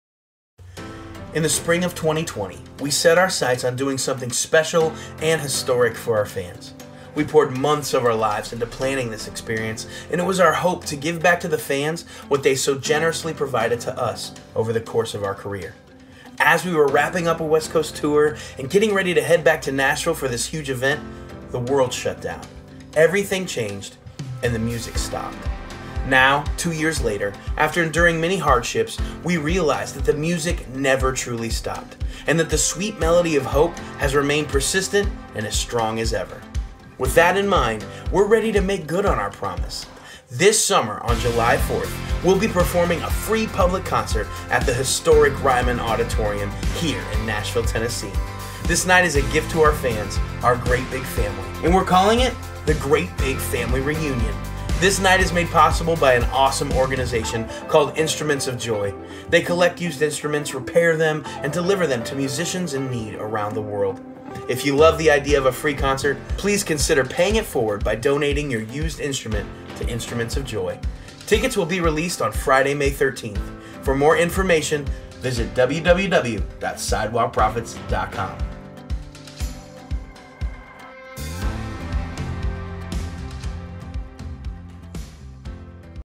Sidewalk Prophets - Great Big Family Reunion 2022 - Ryman Auditorium
Great Big Family,
After two years of waiting, we're finally making good on our promise.  Thanks to our friends at Instruments of Joy,  the "Great Big Family Reunion" is officially happening, and we're gifting YOU a  FREE concert at historic Ryman Auditorium. Doors open at 6pm, and show starts at 7pm Central.  To top it off, the concert is on the 4th of July, which means you can experience the Ryman with us, and then take part in one of the country's largest fireworks shows.

Tickets for this event will be released on Friday, May 13th at 10am central.  This event is free but you will need a ticket to attend.  In order to fend off scalpers, these tickets 1) Are non-transferable. 2) Require an ID to attend.  We're doing everything in our power to make sure these tickets end up in the hands of fans.